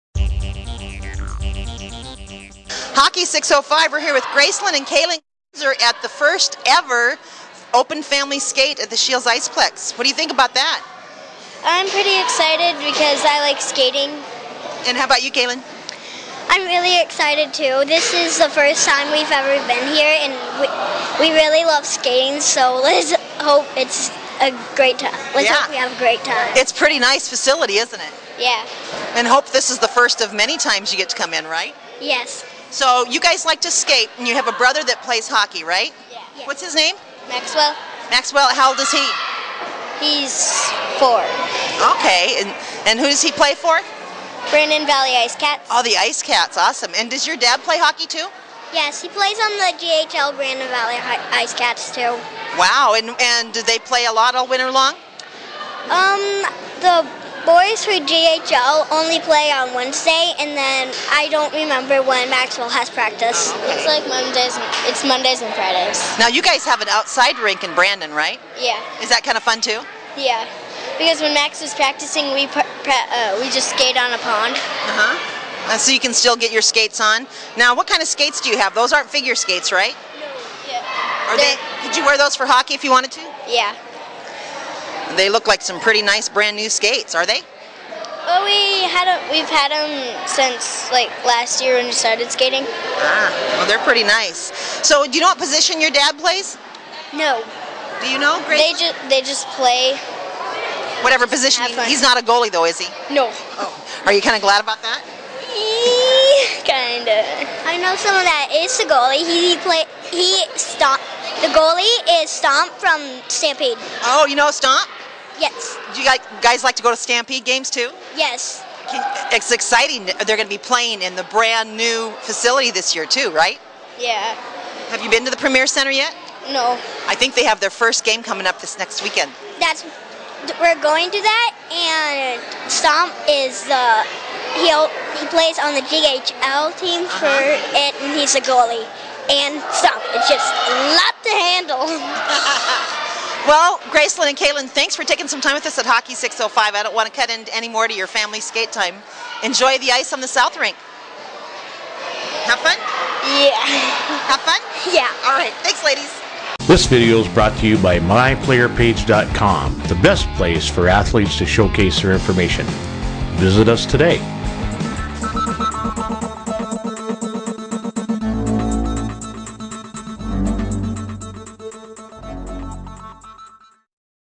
Gracelynn & Kaelynn at Culver's
October 5, 2014 in Sioux Falls, SD - Hockey605 was on hand for the first-ever Culver's Family Open Skate at the brand new Scheels IcePlex, where we met sisters Gracelynn and Kaelynn, and spent a few minutes visiting with them.